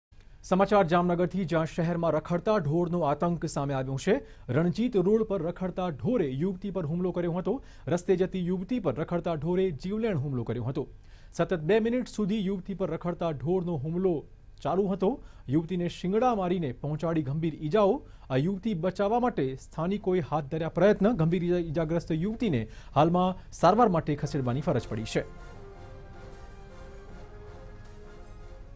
Caught on CCTV: Stray cattle charges woman in Jamnagar, woman hospitalized| TV9News
Caught on CCTV: Stray cattle charges woman in Jamnagar, woman hospitalized| N05